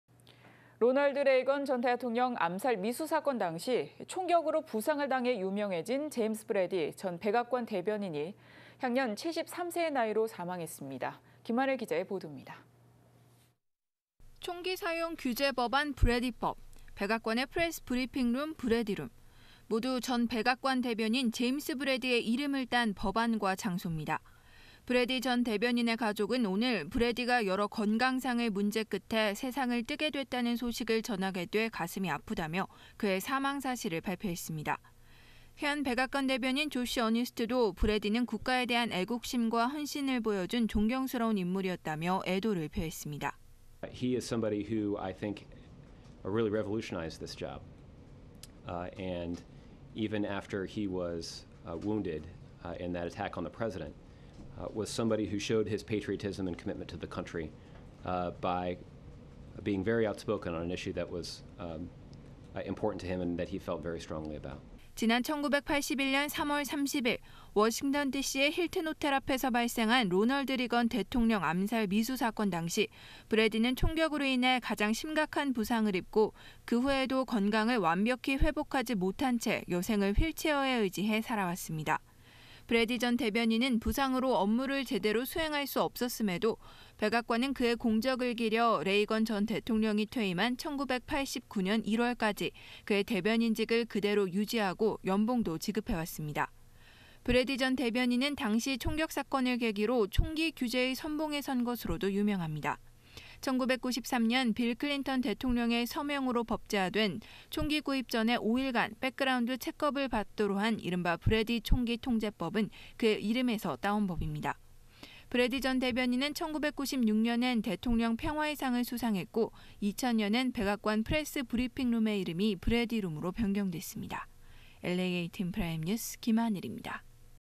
제임스 브래디 전 백악관 대변인 사망
로널드 레이건 전 대통령 암살미수 사건 당시 총격으로 부상을 당해 유명해진 제임스 브래디 전 백악관 대변인이 향년 73세의 나이로 사망했습니다. 김하늘 기자의 보돕니다.